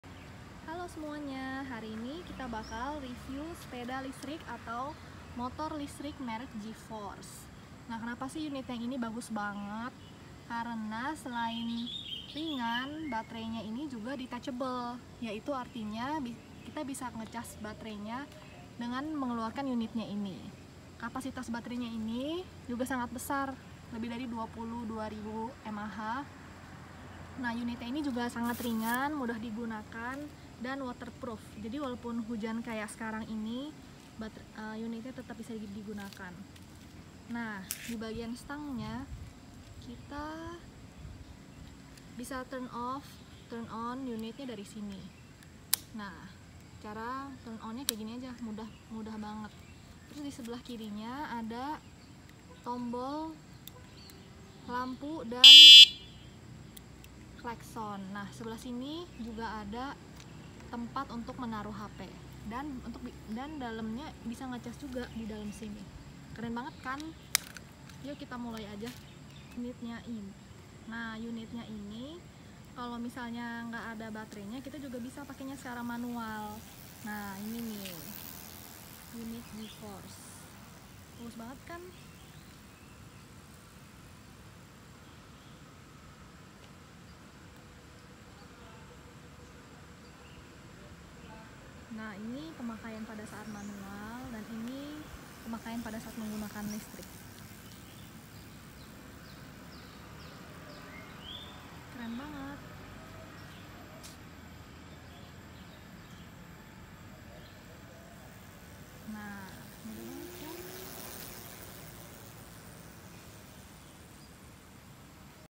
Review Sepeda Listrik Lipat Terbaik di INDONESIA!!
Kapasitas baterai 22800 mAH
Waterproof
Baterai bisa di charge dengan dilepaskan dari unit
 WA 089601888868